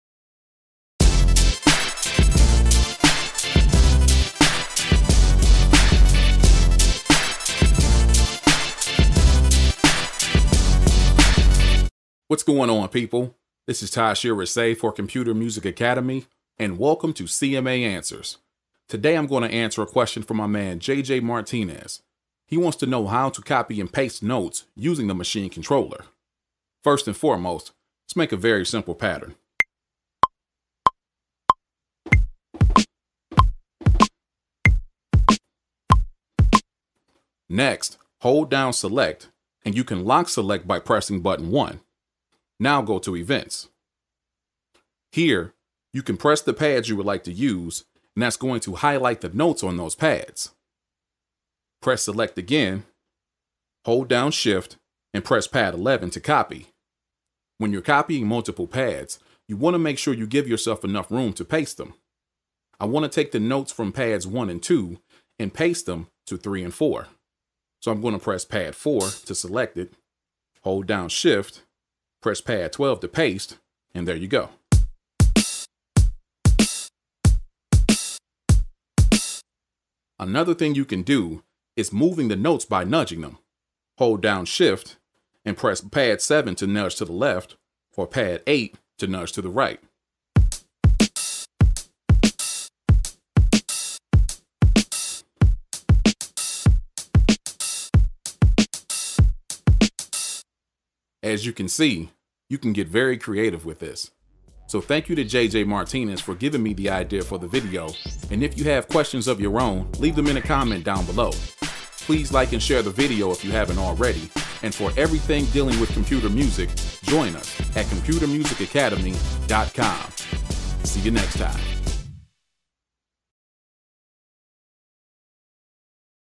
Native Instruments Maschine: Copy & Paste Notes | Answers | Computer Music Academy
In this video I'm responding to JayJae Martinez. He's a Maschine user and would like know who to copy and paste notes by using the controller and not the mouse. This is actually a very simple process but of course I give you a little something extra. If you have questions of your own, leave them down below in the comment section.

For more on Maschine and everything dealing with Computer Music, Join Us!